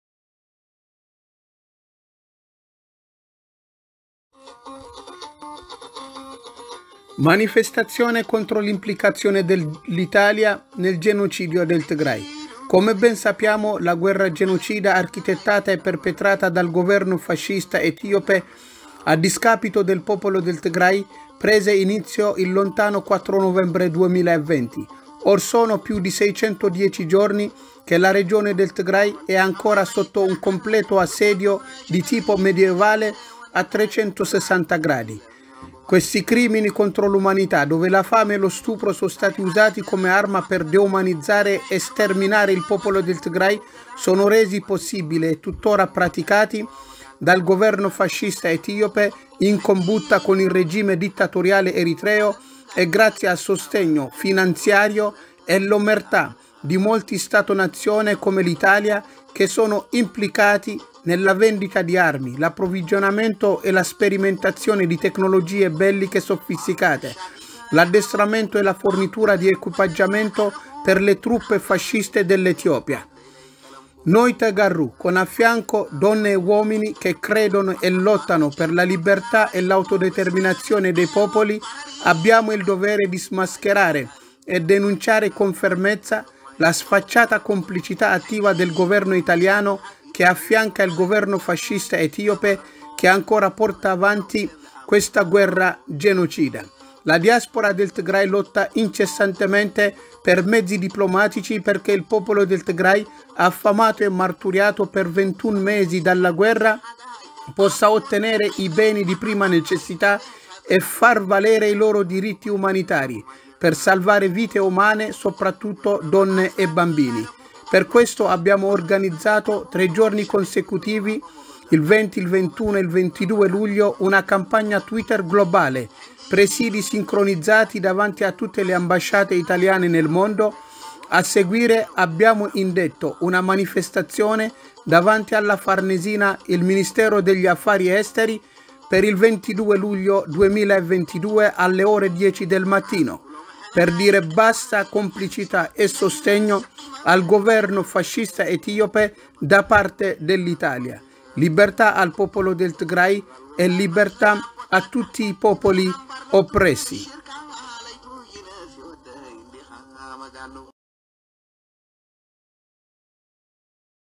Tigray, manifestazione globale contro il supporto dell' Italia al "governo genocida etiope"
Venerdì 22 luglio 2022 dalle ore 10.00 ci sarà un presidio davanti alla sede della Farnesina, Ministero degli Affari Esteri a Roma.